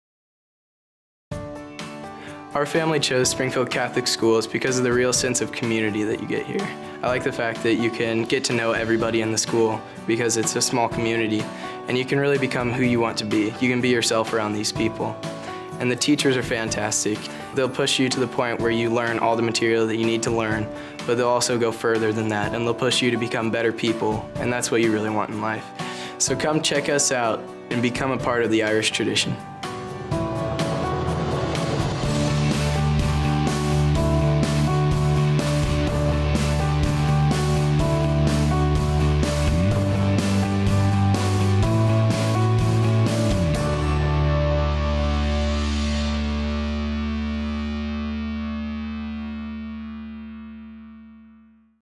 2014 Springfield Catholic "We Are Springfield Catholic!" - Short - 1080p by Snobl Productions
We are Springfield Catholic!  Ben Bowman describes why he and his family chose to attend Springfield Catholic Schools.